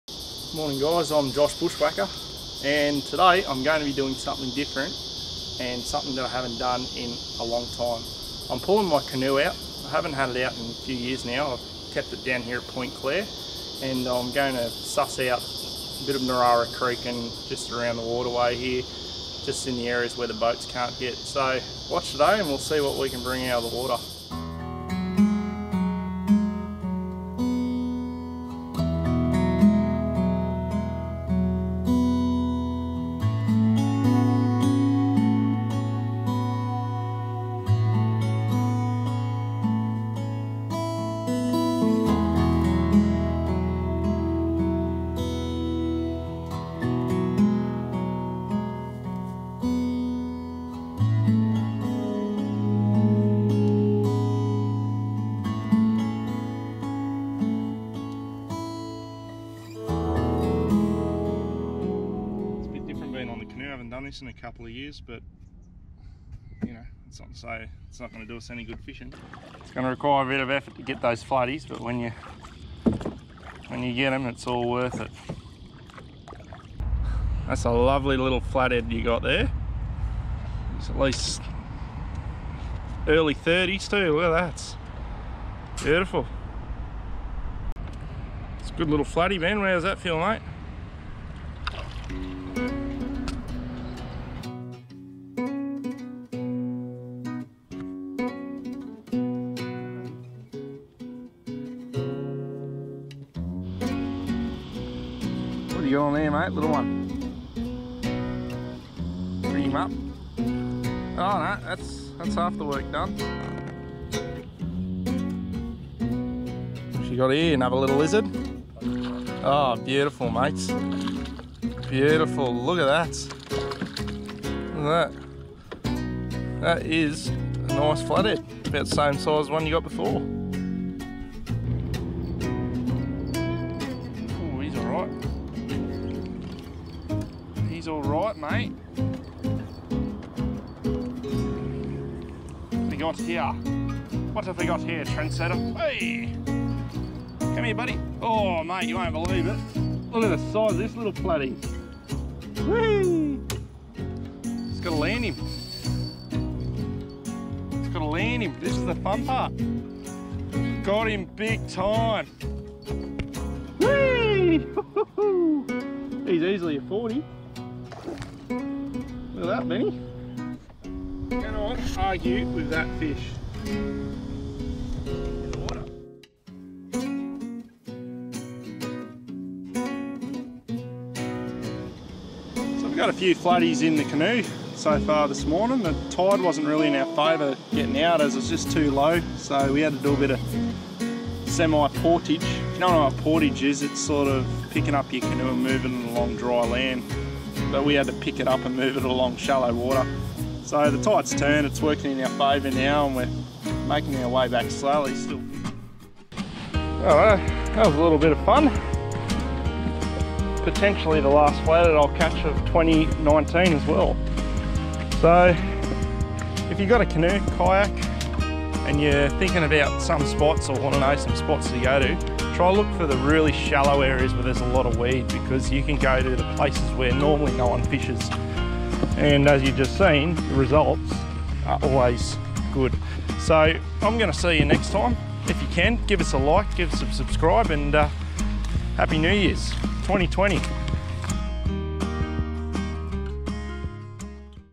Dusky flathead near Gosford
It is summer and I found opportunity to use my Wenonah Canoe. It has been collecting dust for years but my self and my brother launched it in some shallow water around west Gosford and paddled up Narara Creek looking for some Flathead.